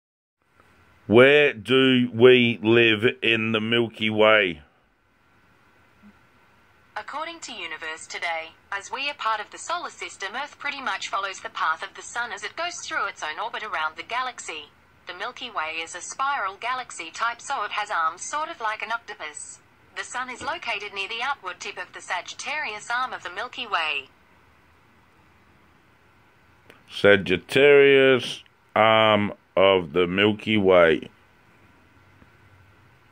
MANDELA EFFECT- ASK GOOGLE WHERE DO WE LIVE IN THE MILKY WAY ( SAGITTARIUS SUN?) AUDIO EVIDENCE.
Same search but proven with live capture incase it vsnishes in the future. Cheers all.